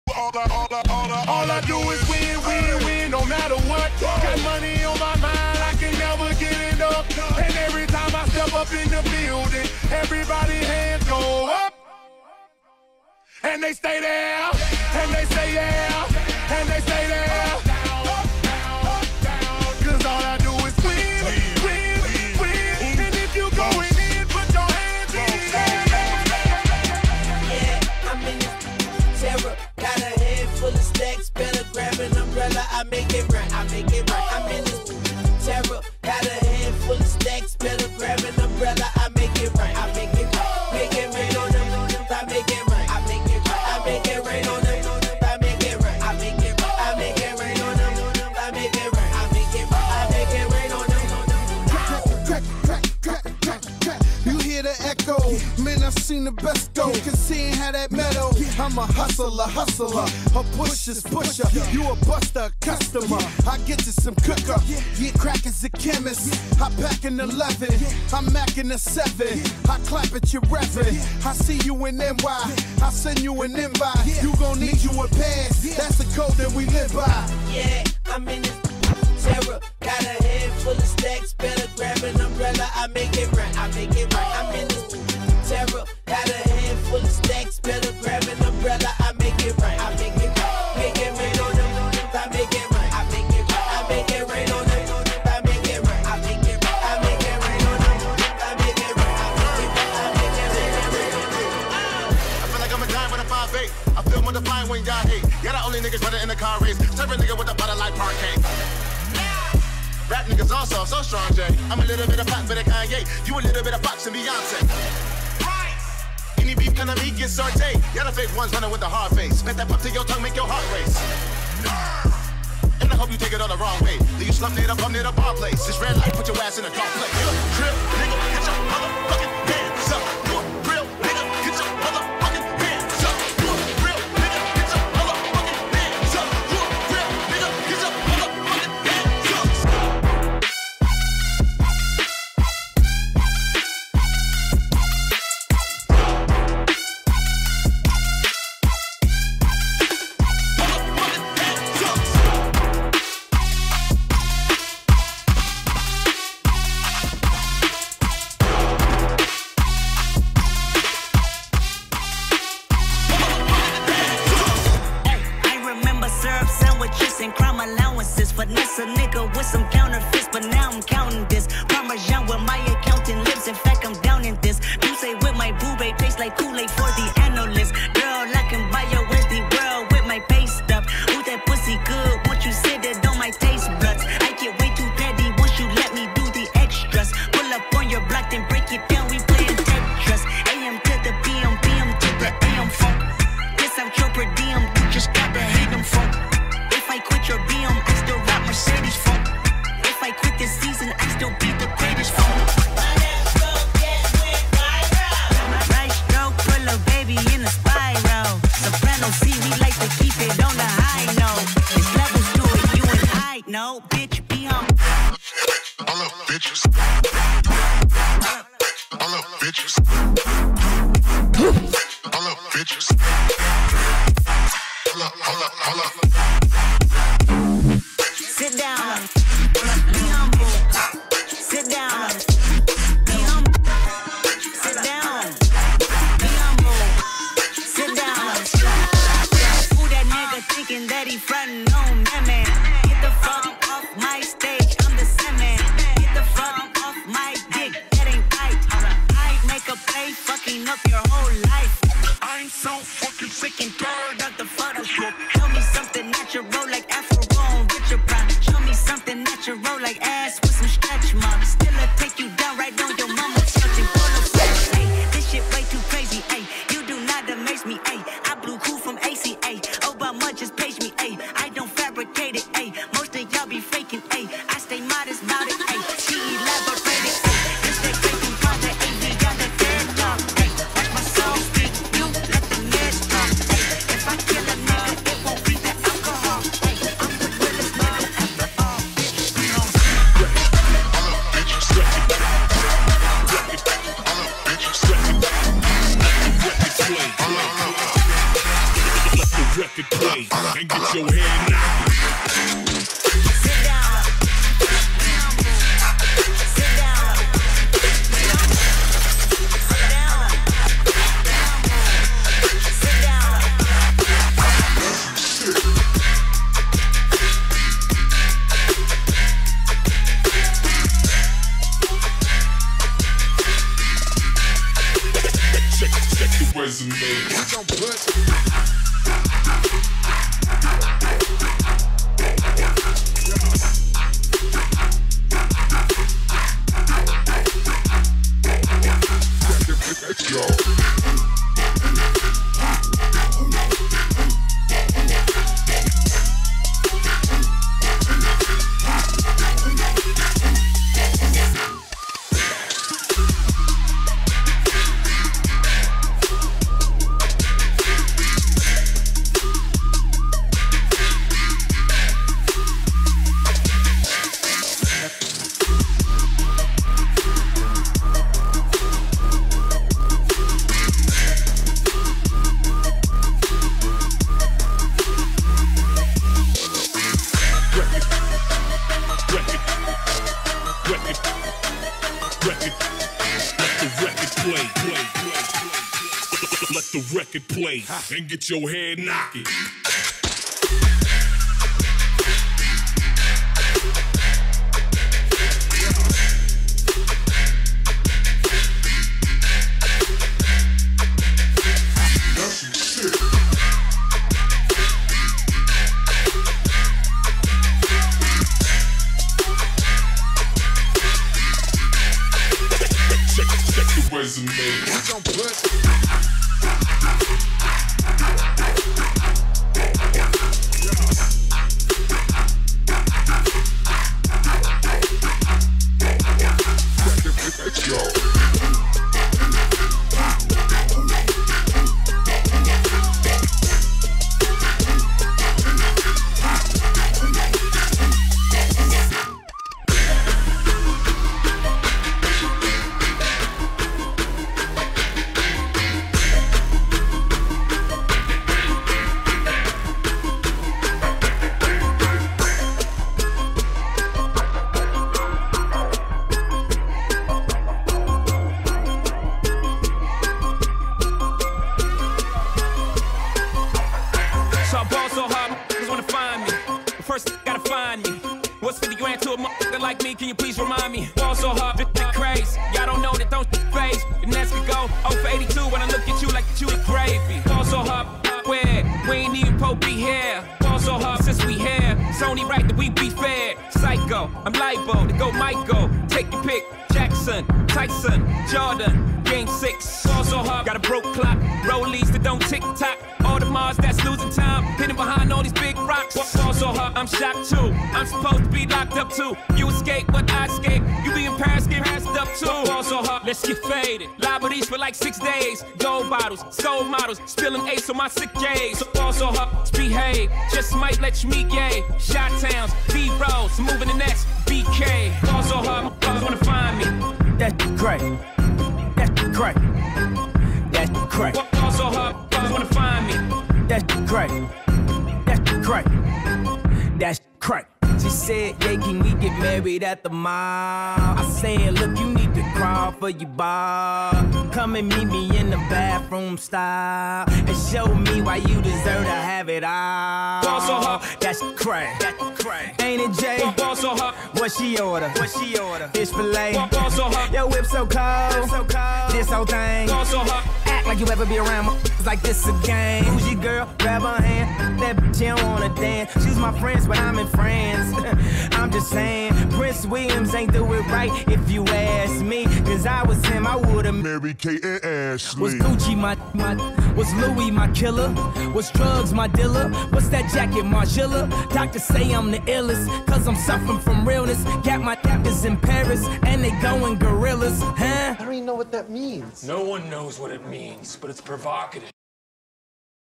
Live DJ Mix with Visuals #70: Hip Hop, Trap
Hip Hop, Trap music mix excerpt from DJ Derek Sav's Saturday night VJ/DJ set at Logan's Beach Bar, Hollywood Beach, Florida.